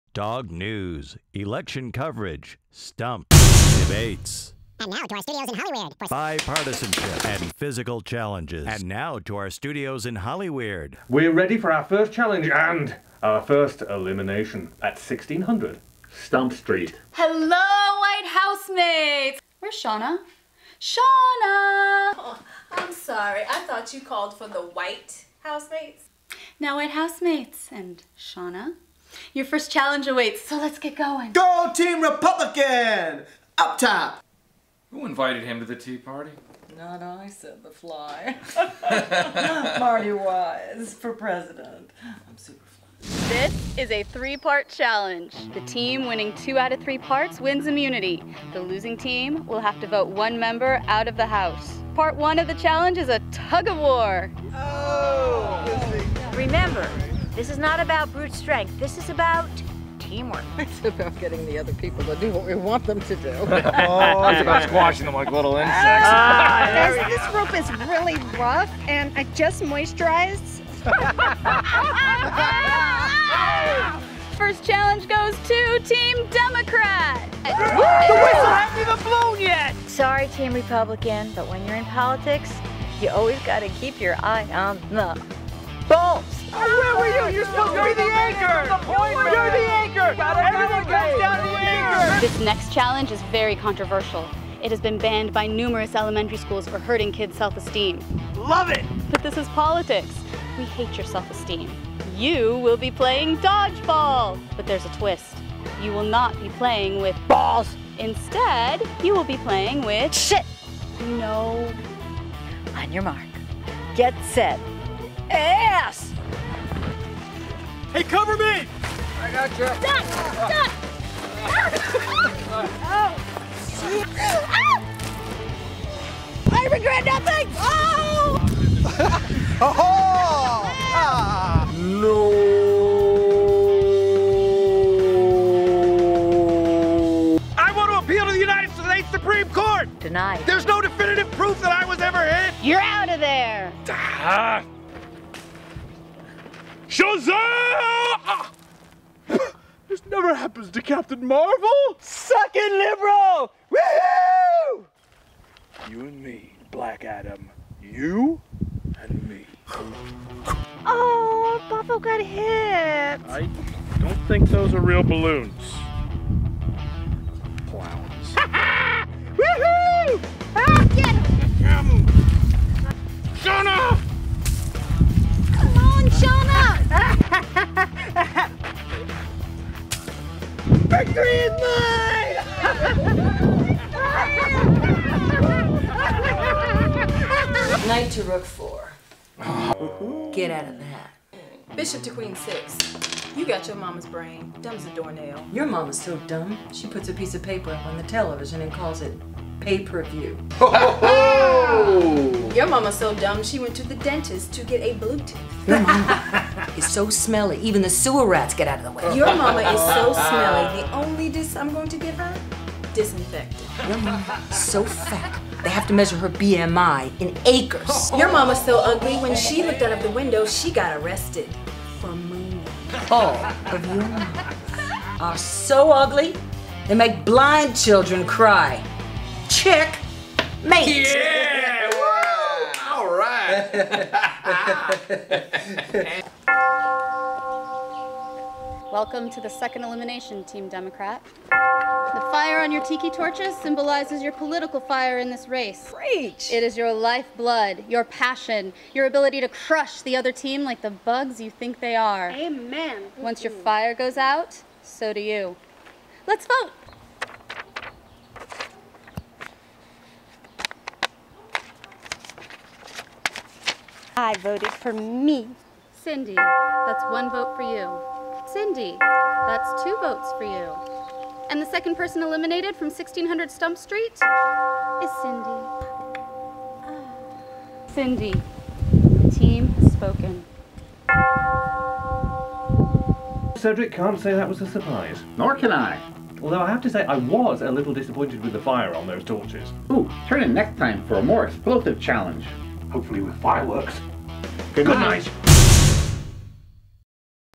STUMP: The First Challenge
The "White Housemates" engage in their first challenge. Who wins and who is ultimately eliminate from 1600 Stump Street?